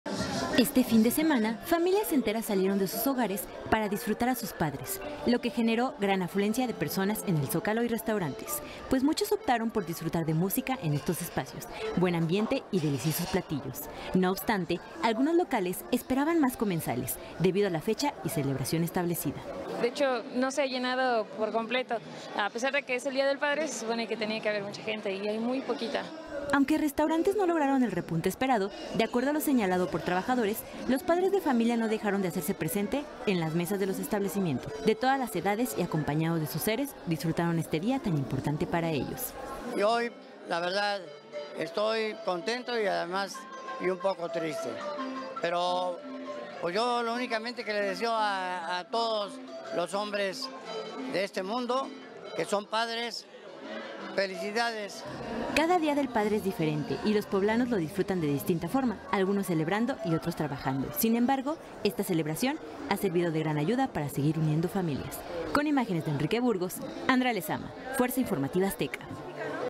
Celebran día del padre
Cientos de poblanos celebraron a papá en diversos comercios de la capital poblana